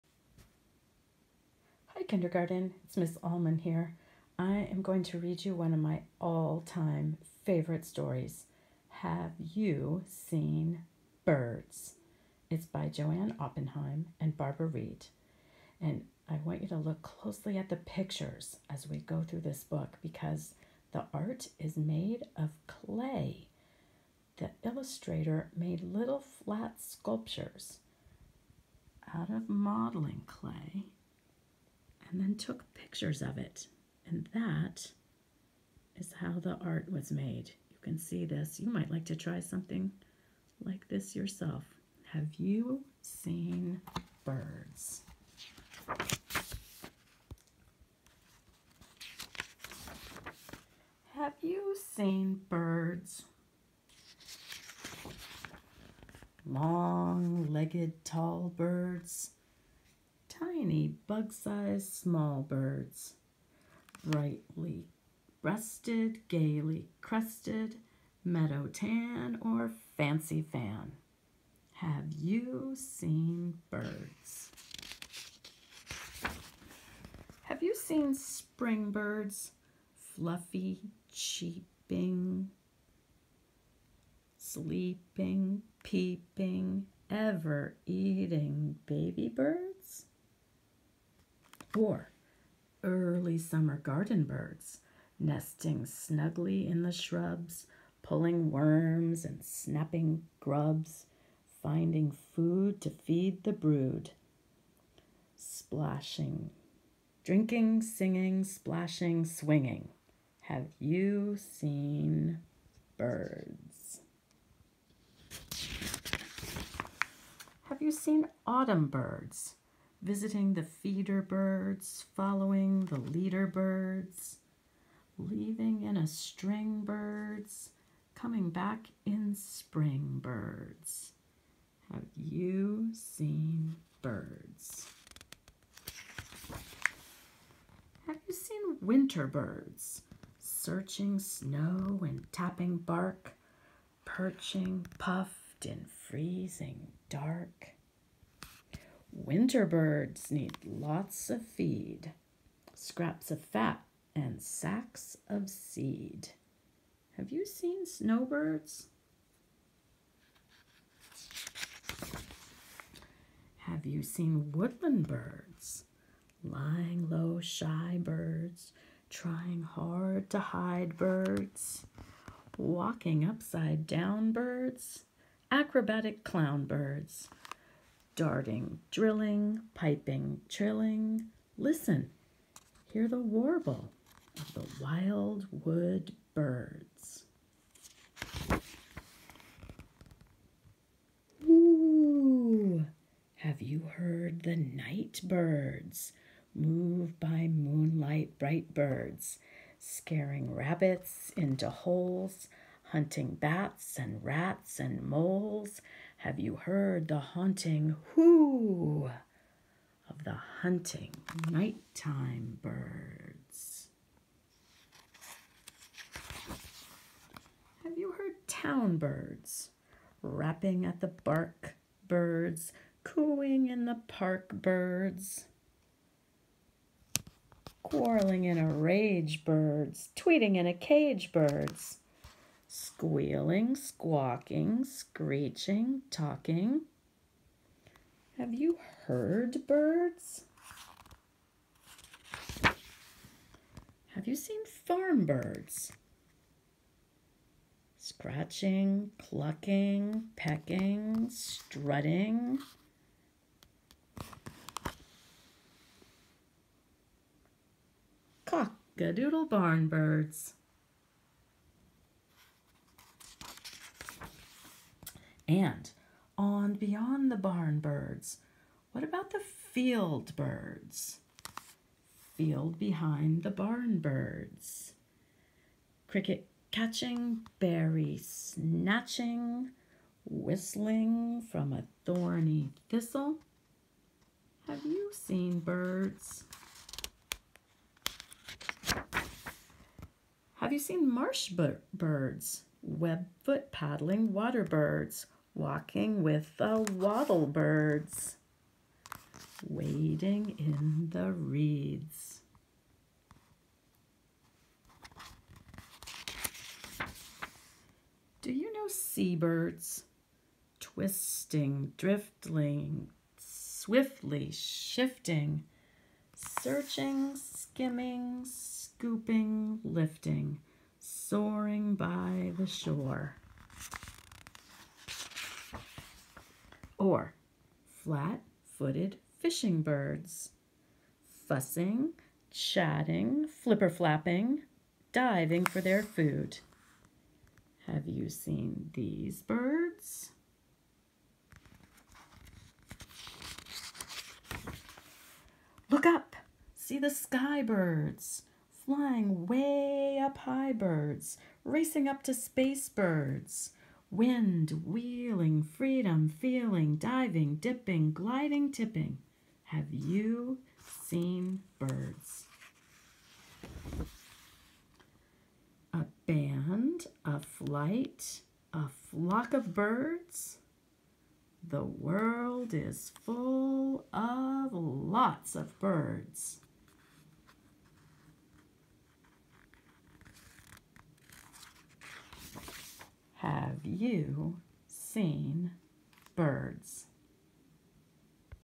Have You Seen Birds?
Ms. Allman reads one of her favorite books.